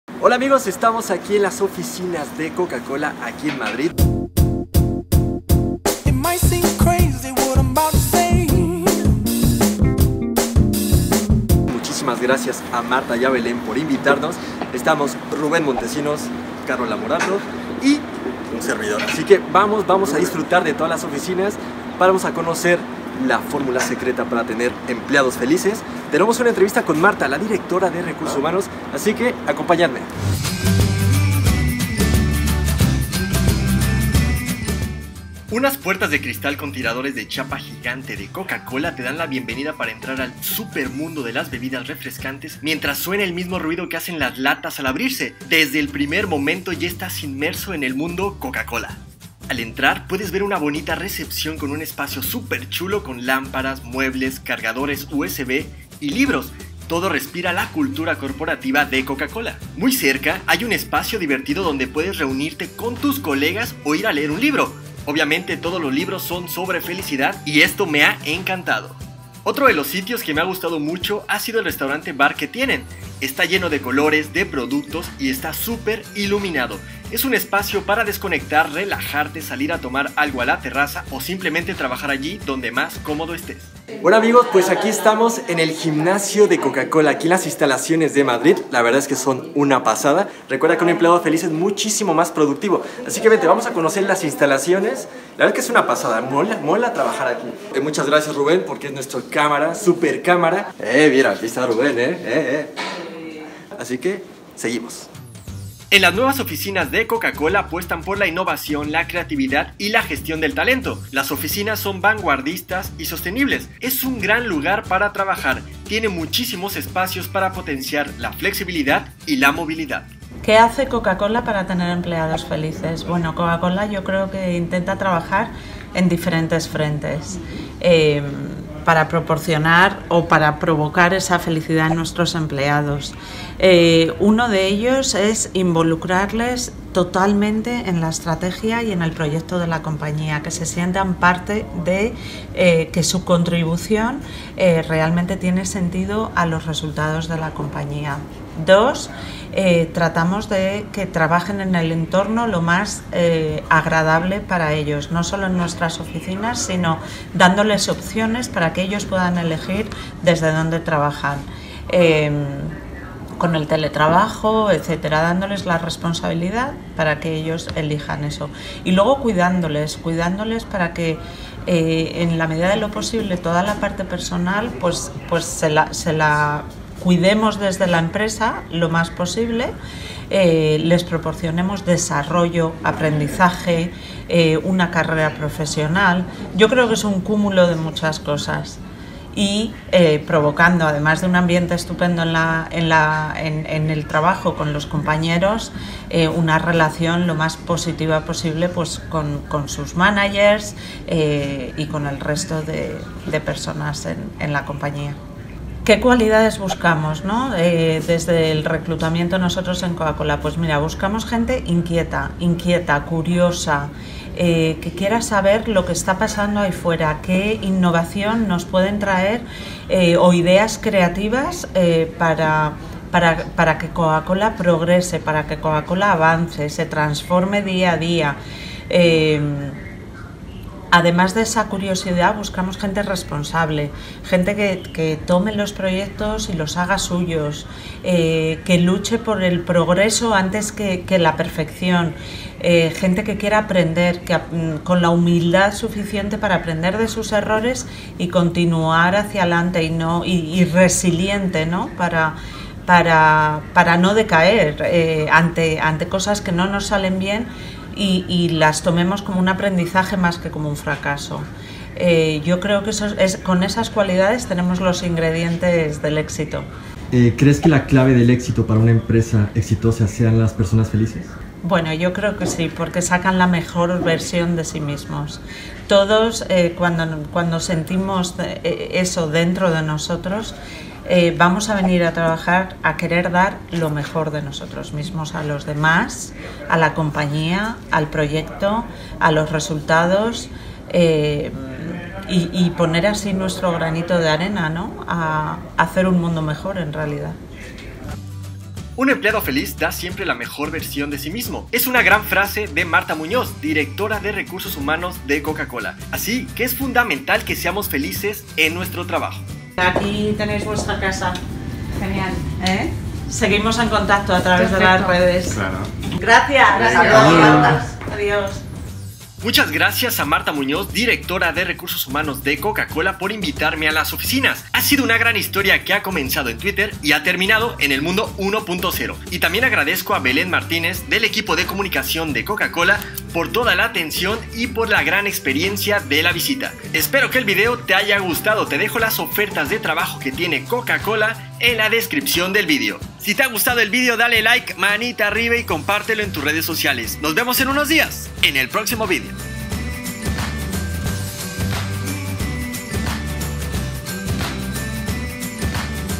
Conoce las oficinas de Coca Cola España 😃 Entrevista a Marta Muñoz Dira. Recursos Humanos
¡Conoce las oficinas de Coca Cola España!
Descubre cómo es trabajar en Coca-Cola y cómo tener empleados felices con la entrevista a Marta Muñoz Directora de Recursos Humanos de Coca-Cola Iberia.

He disfrutado del tour en las oficinas junto a Ruben Montesinos y a Carola Morato.Muchas gracias a Marta Muñoz, Dira. de RRHH y a Belen Martinez, Consumer Interaction Center Manager de Coca-Cola, por la fantástica experiencia en las oficinas.

Una persona feliz en su trabajo es mucho más productiva 😃

▂ ▃ ▅ ▆ █ SÍGUEME █ ▆ ▅ ▃ ▂